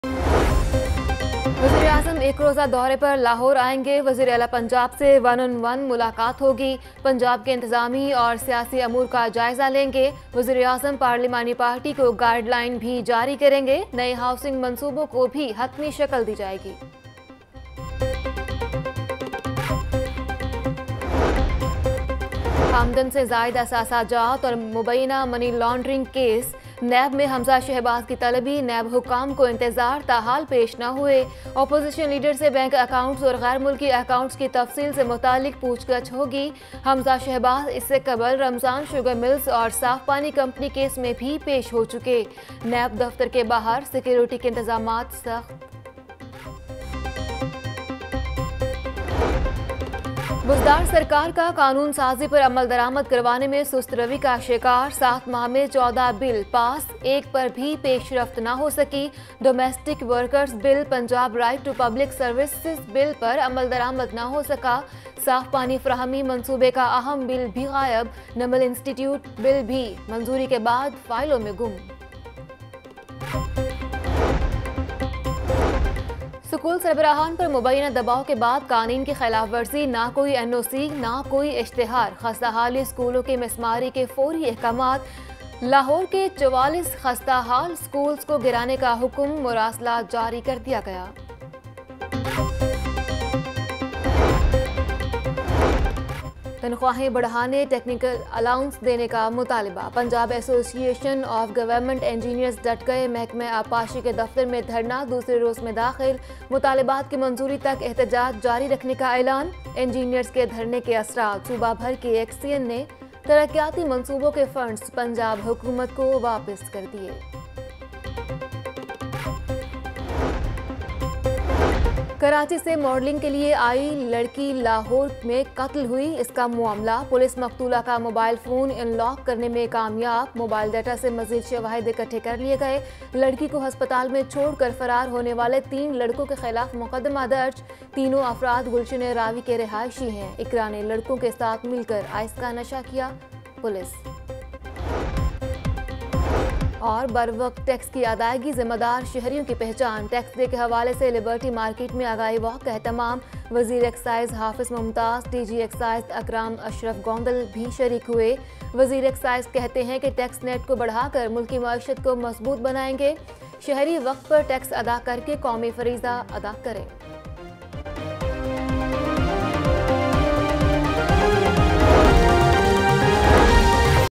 NAB summons Hamza Shahbaz in Money Laundering Case | 11 AM Headlines - 10 April | Lahore News HD
Watch Latest News:
Shehbaz Sharfi leaves for London to see his grandchildren, Prime Minister Imran Khan will visit Lahore today,  NAB Court indicts Shahbaz Sharif and Hamza Shahbaz in Ramzan Sugar Mills Case, Hamza Shahbaz will appear before NAB in money laundering case, Education Dept orders to demolish 44 school buildings, Husband shaves off wife’s hair for refusing to dance before friends , Nawaz Sharif released on six weeks bail,  LHC removed Shahbaz Sharif’s name from ECL, Police suspends 36 DSP’s , PM Imran Khan expresses dismay over Punjab MPAs' salary increase, CM Punjab's salary soars high following recent salary increments, Over 34770 cars found registered in name of one citizen .